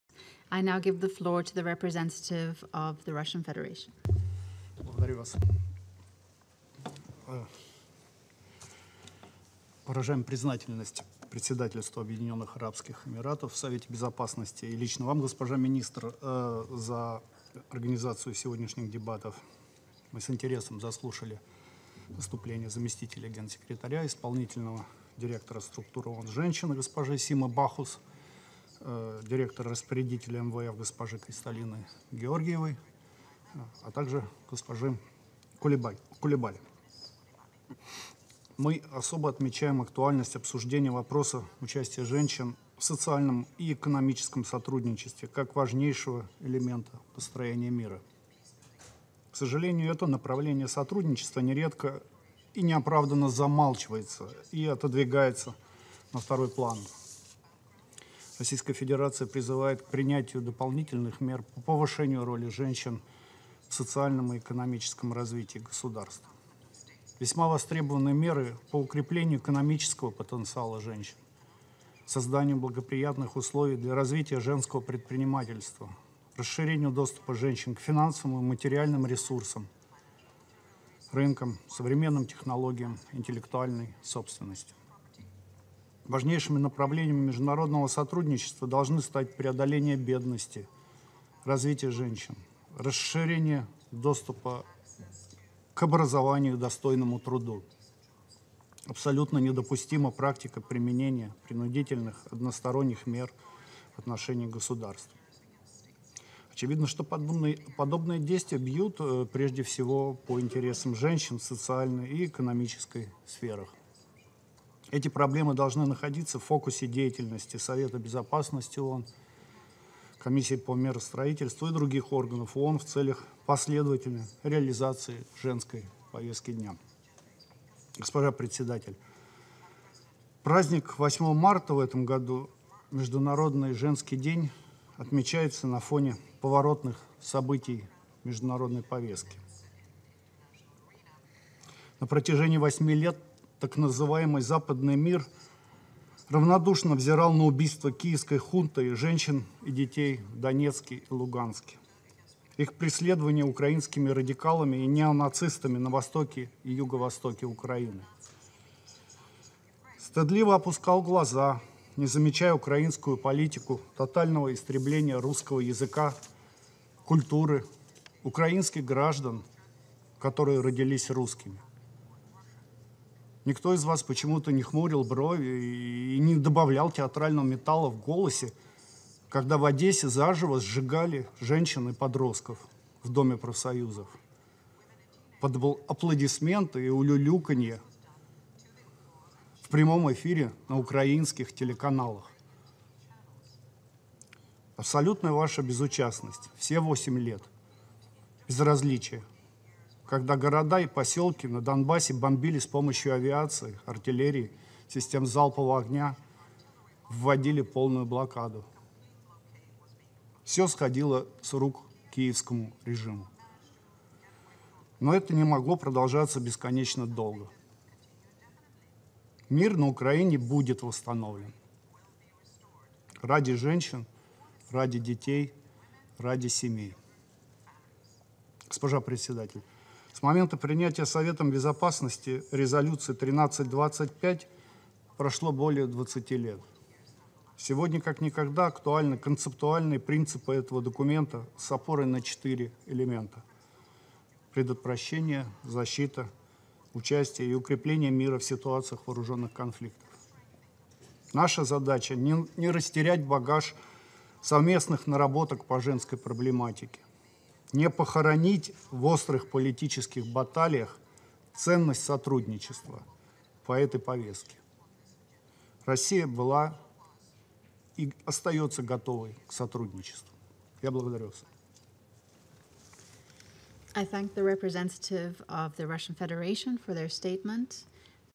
Выступление Г.В.Кузьмина на заседании СБ ООН по тематике женщин, мира и безопасности
Выступление заместителя Постоянного представителя Г.В.Кузьмина на заседании СБ ООН по тематике женщин, мира и безопасности, 8 марта 2022 года